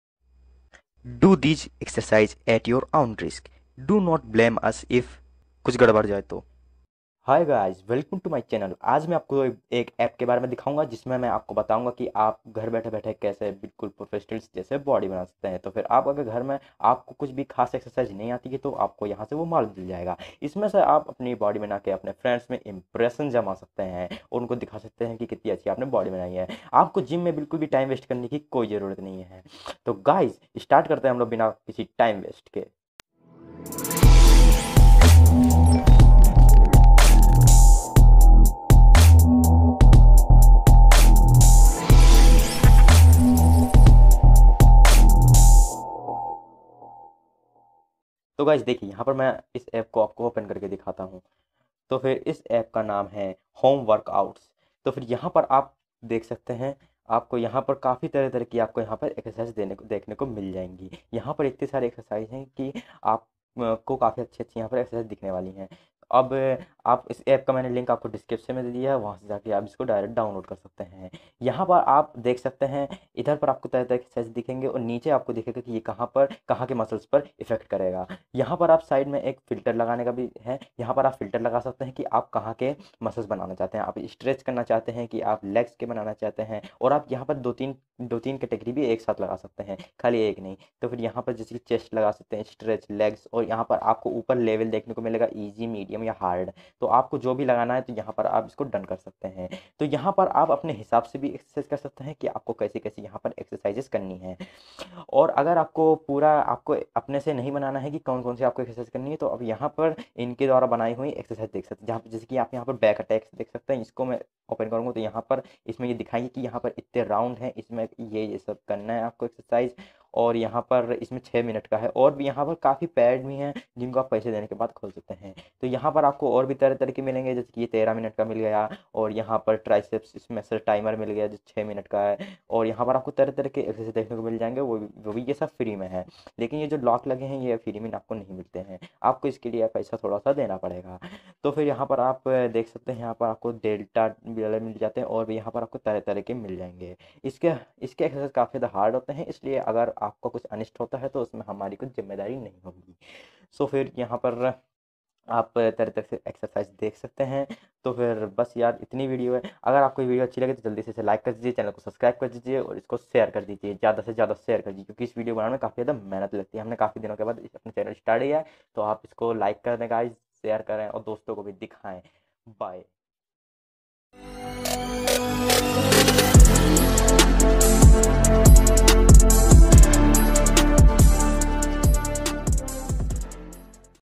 Best fitness app for android/how to build muscle at home/bodybuilding at home in hindi/easy exercise
Easy exercise at home is here...
In this video you can see an amazing android fitness app.
Which is paid in playstore.